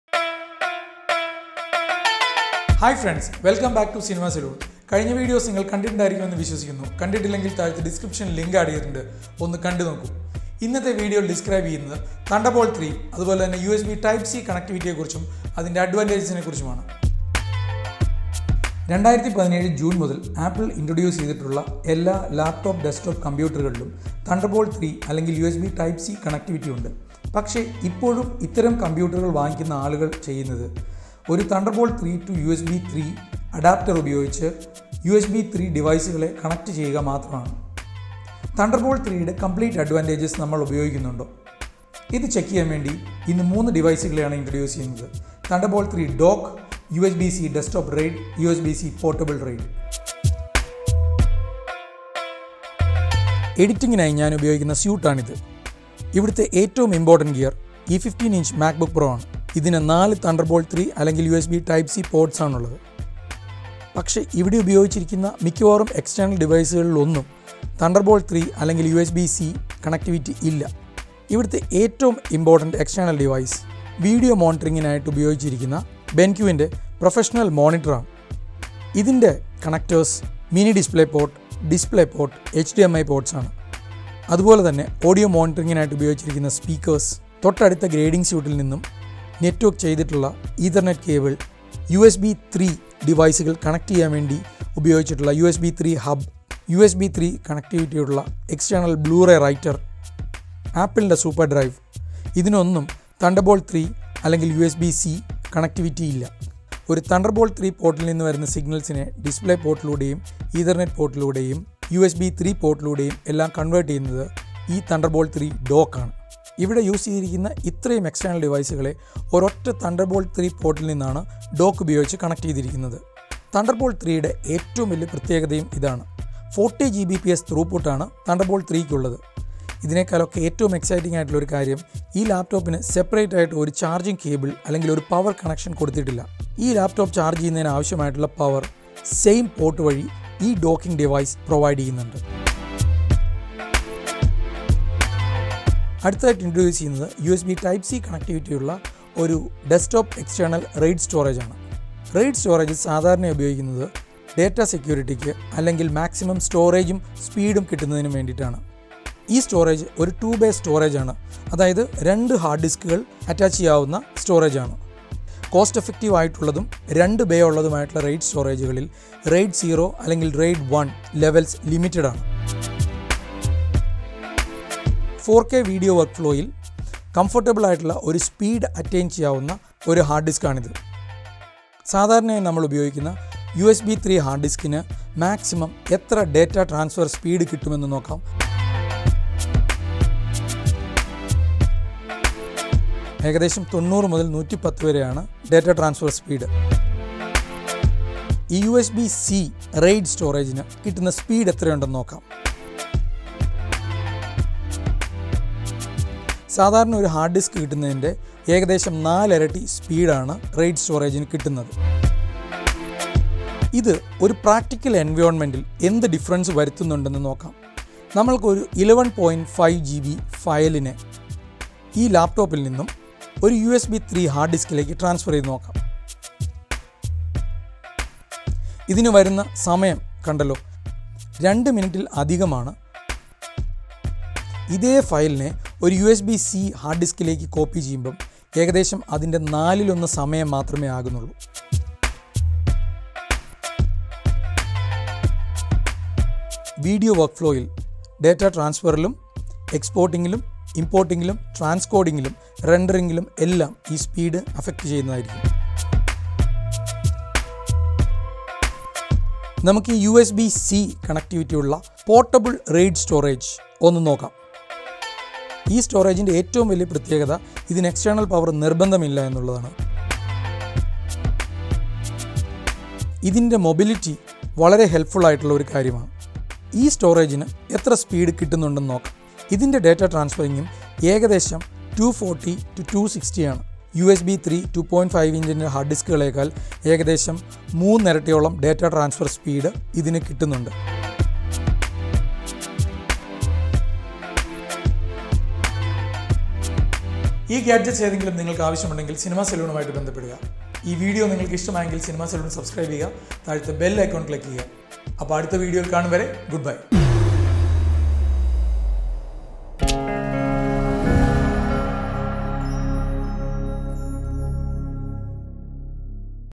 Thunderbolt 3 / USB C Connectivity (മലയാളം)
Sreekumar Nair, Colourist & Apple Certified Pro, Cinema Salone, explains about Thunderbolt 3 & USB Type C connectivity.
Thunderbolt 3 price : MRP Rs18,625